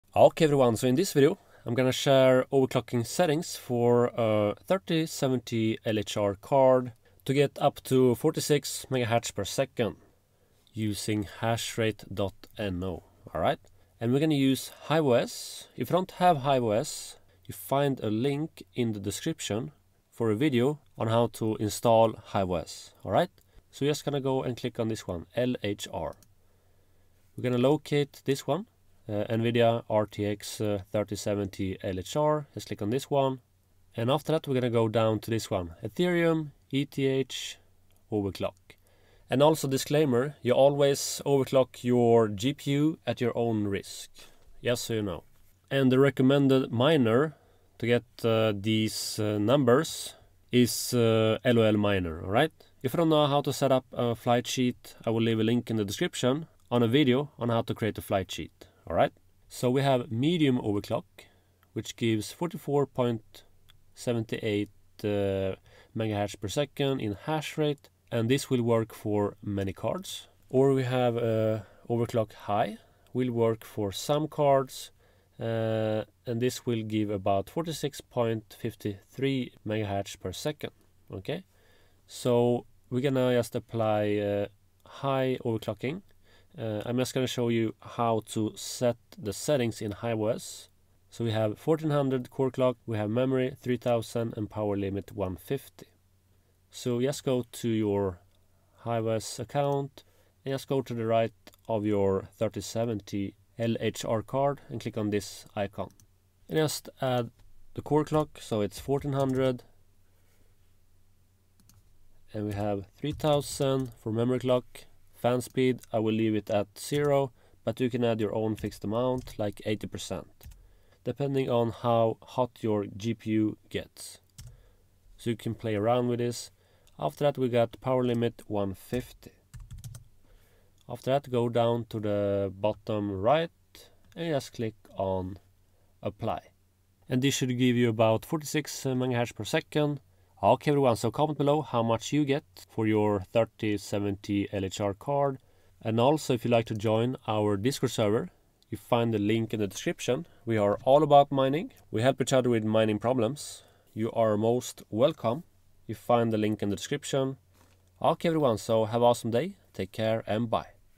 46MH/s++ RTX 3070 LHR Unlock Overclock Settings in HiveOS With LolMiner
RTX 3070 LHR Overclocking setting that gives up to 46++MH/s hashrate with LolMiner LHR Unlocker in HiveOS.

We use hashrate.no to get the overclocking setting for this 3070 LHR card. They have medium and high overclocking settings that you can apply in HiveOS to get between 44-46MH/s in hashrate for algorithm Ethash when mining Ethereum.